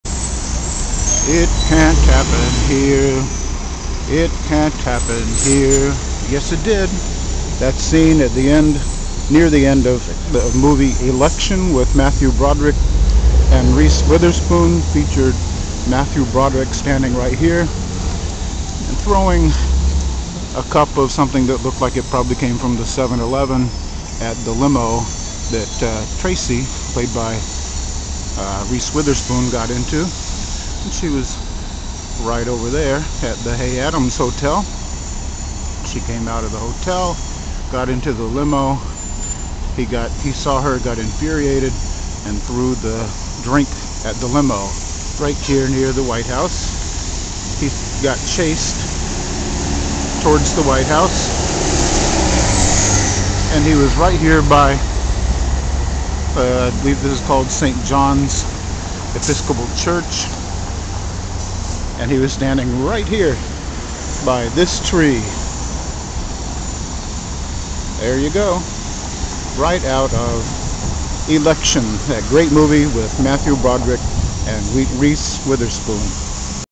DC Films: "Election" scene site, the Hay Adams Hotel
Washington DC. Here's where that hilarious scene near the end of the movie "Election" took place, in which Matthew Broderick hurls a Slurpee or some such drink at the limo that Reese Witherspoon just got into.  It's at 16th and H Streets, by the Hay Adams Hotel and the St. John's Episocopal Church, Washington DC. Barack Obama and family are living in the Hay Adams Hotel for approximately 2 weeks before his inauguration in January 2009.